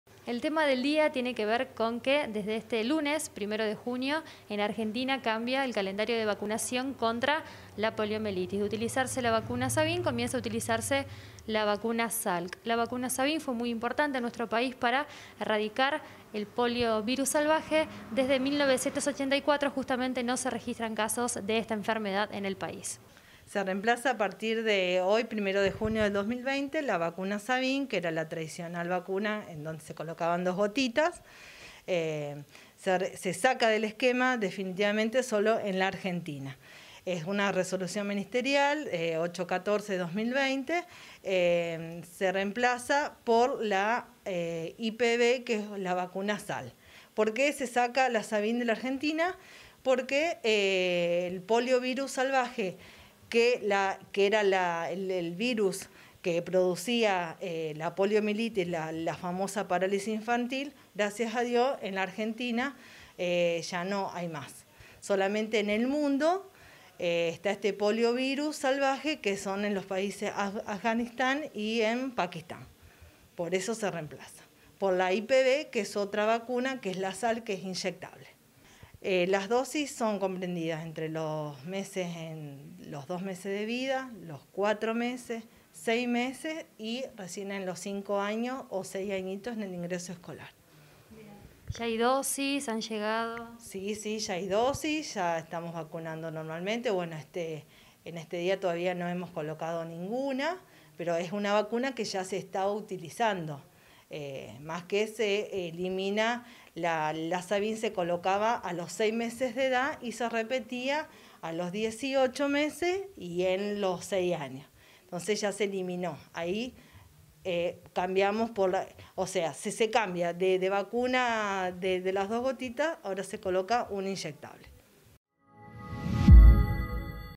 Se dejará de aplicar en todo el país la vacuna oral Sabin contra la poliomielitis
A partir de este lunes, 1 de junio, se dejará de aplicar en todo el país la vacuna oral Sabin contra la poliomielitis y sólo se utilizará la inyectable tipo Salk. Se trata de una vacuna más completa y con menos posibilidades de generar complicaciones derivadas del propio virus.